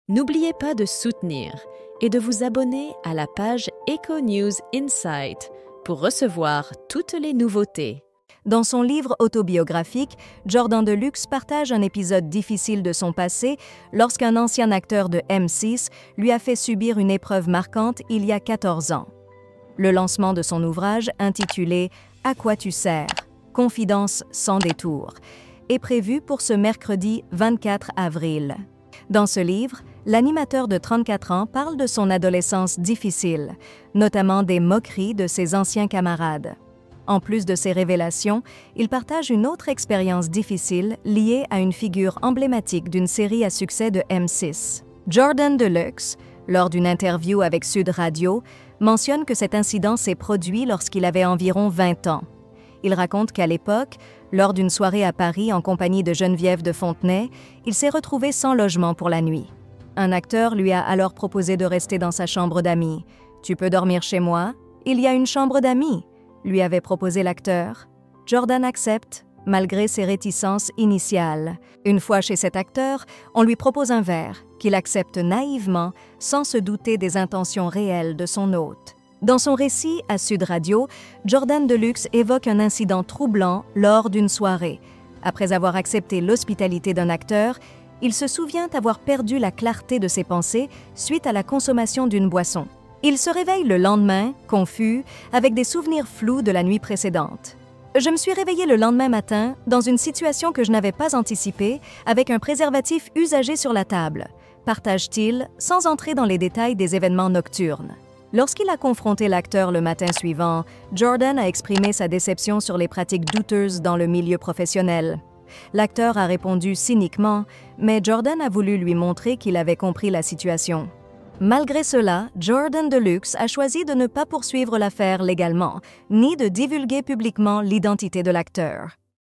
Jordan De Luxe révèle une dure nuit avec un acteur de M6
Dans une révélation choc, Jordan De Luxe partage les détails d'une nuit difficile qu'il a passée avec un célèbre acteur de M6. Dans son dernier entretien, il décrit comment une soirée apparemment banale s'est transformée en une expérience troublante qui l'a marqué. Jordan raconte comment il a été invité à passer la nuit chez cet acteur après une soirée ensemble, et les événements qui ont suivi. Sans entrer dans les détails sensibles, il exprime les difficultés qu'il a rencontrées et comment cet incident a impacté sa vie personnelle et professionnelle.